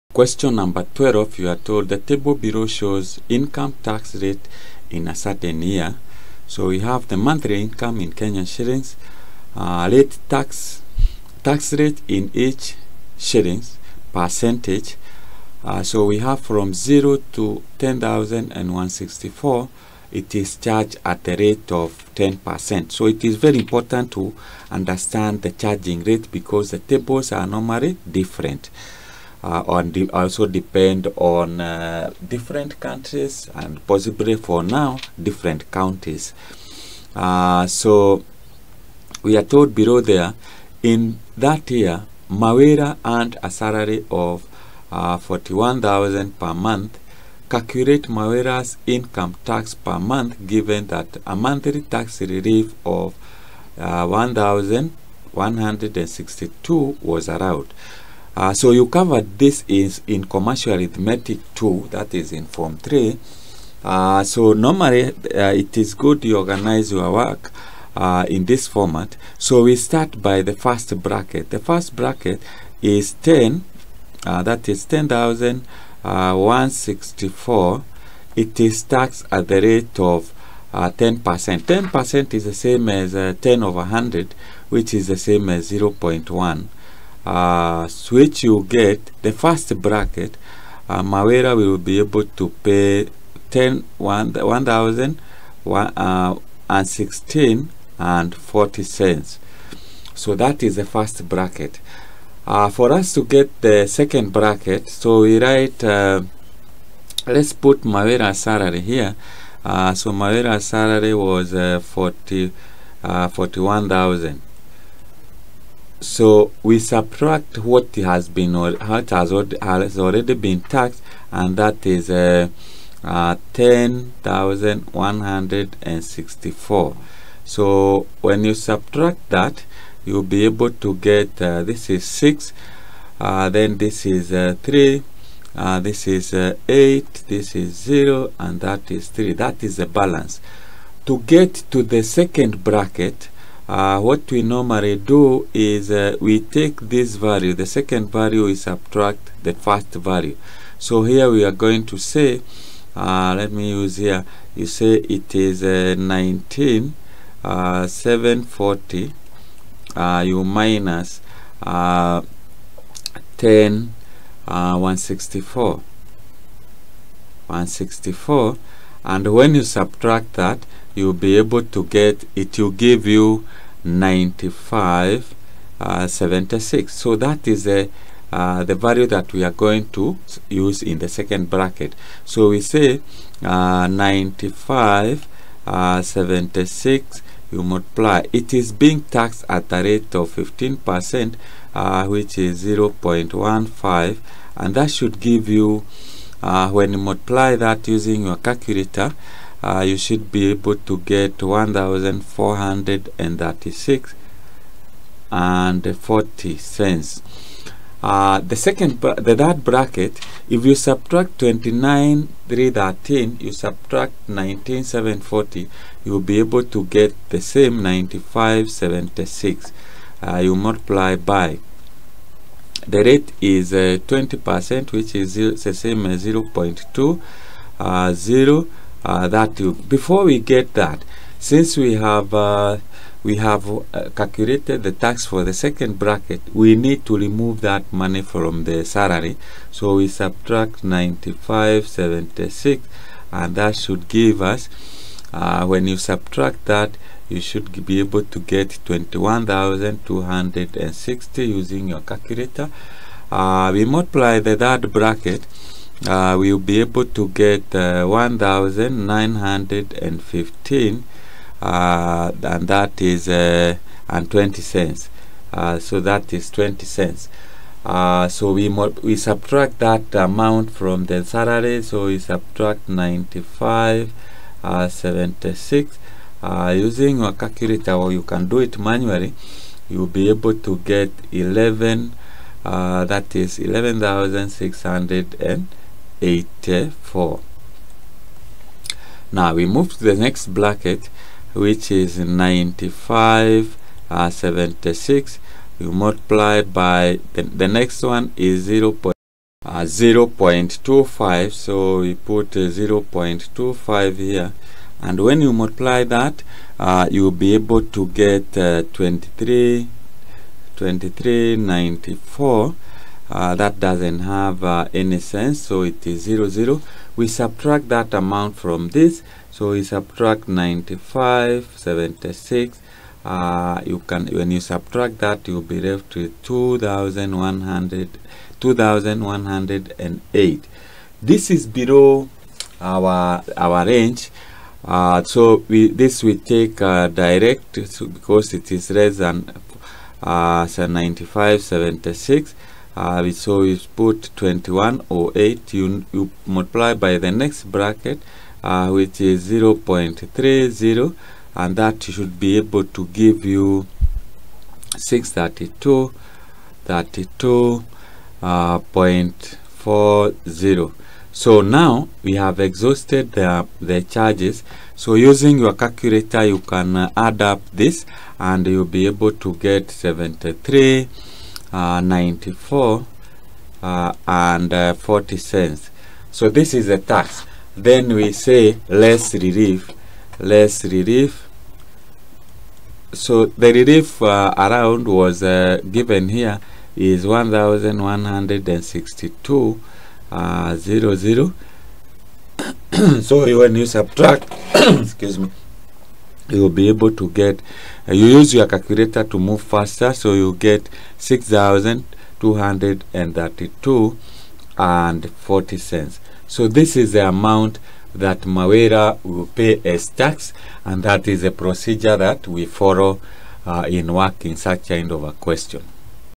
Question #12 2019 KCSE Maths Paper 2
This video explains how to answer Question #12 on the 2019 KCSE Mathematics Paper 2